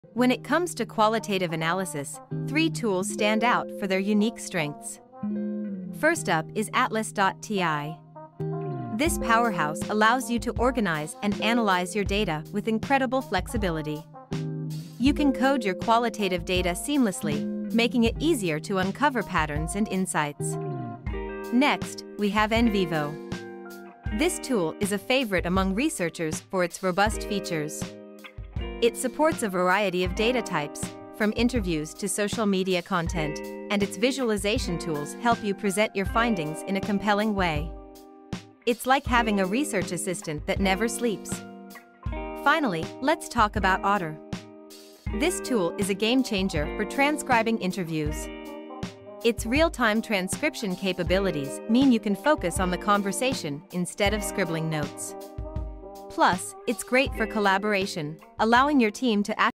Top 3 tools and software for qualitative data analysis
In this video i have explained three important tools for qualitative analysis.